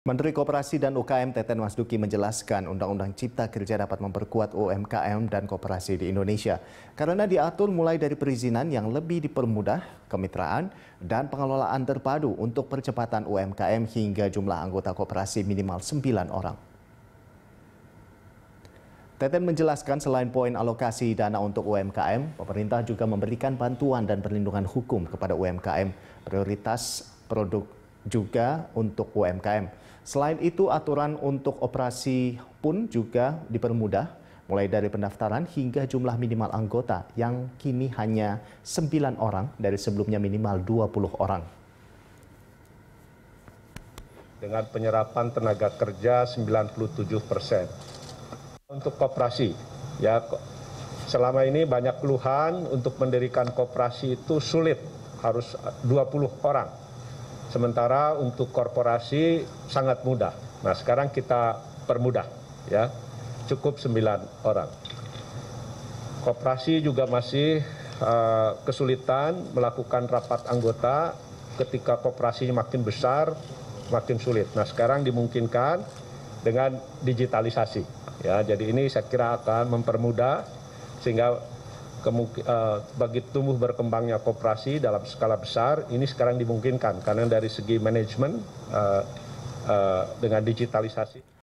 Teten Masduki: UU Cipta Kerja Perkuat UMKM dan Koperasi
Menteri Koperasi dan UKM Teten Masduki menjelaskan Undang Undang Cipta Kerja dapat memperkuat UMKM dan koperasi di Indonesia. Karena di dalamnya mengatur mulai dari perizinan yang lebih dipermudah, kemitraan, dan pengelolaan terpadu, untuk percepatan UMKM, hingga jumlah anggota koperasi minimal 9 orang.